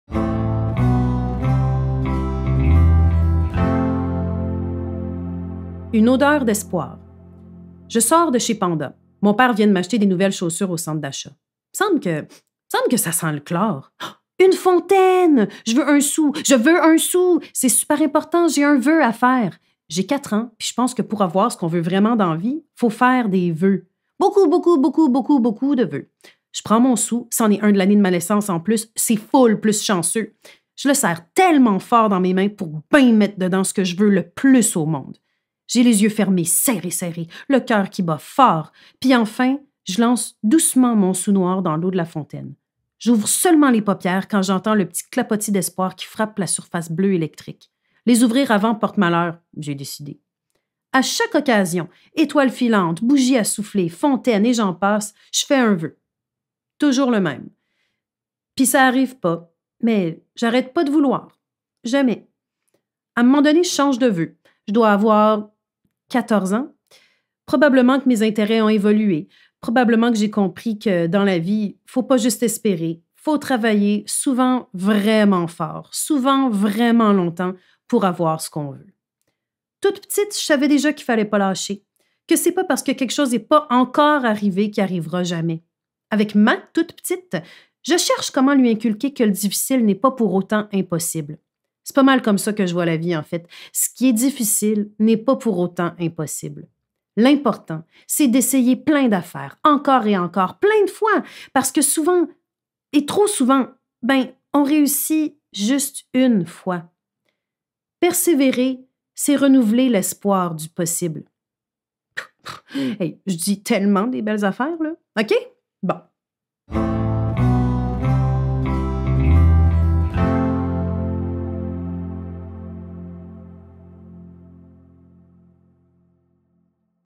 La bulle de MS E05 - Une odeur d'espoir...
La Bulle, c'est une interprétation ludique du thème hebdomadaire du balado À la hauteur de nos tout-petits de Meggie Bélanger. L'autrice MS Bérard y livre un conte intimiste où la perspective de l'enfance trouve sa place dans le monde adulte de la maternité.